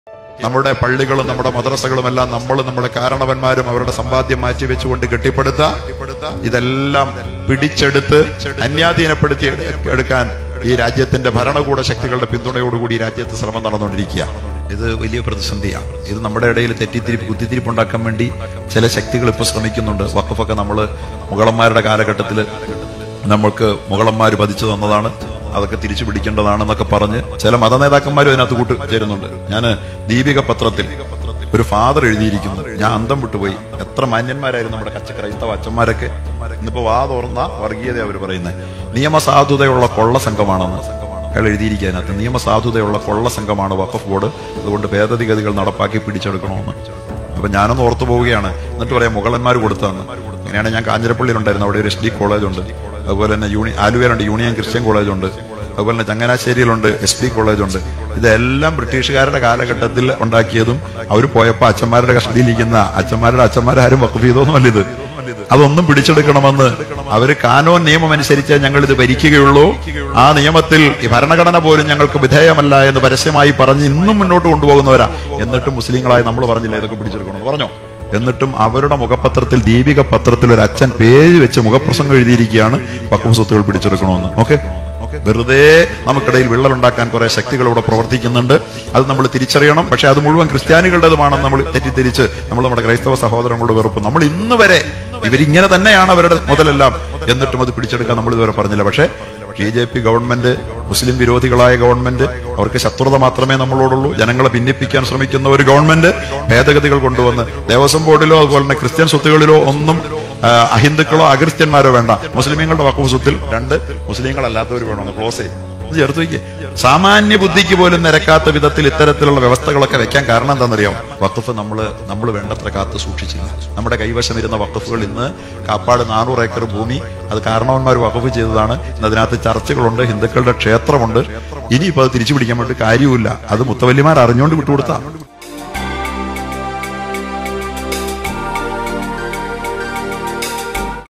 ദീപിക പത്രത്തിലെ ജോഷിയച്ചന്റെ ലേഖനത്തിനെതിരെ ഉസ്താദിന്റെ പ്രസംഗം |PRIEST|USTHAD SPEECH| GOODNESS TV
YouTube Channels
Mobile Apps:
►Goodness TV:
►Goodness Radio: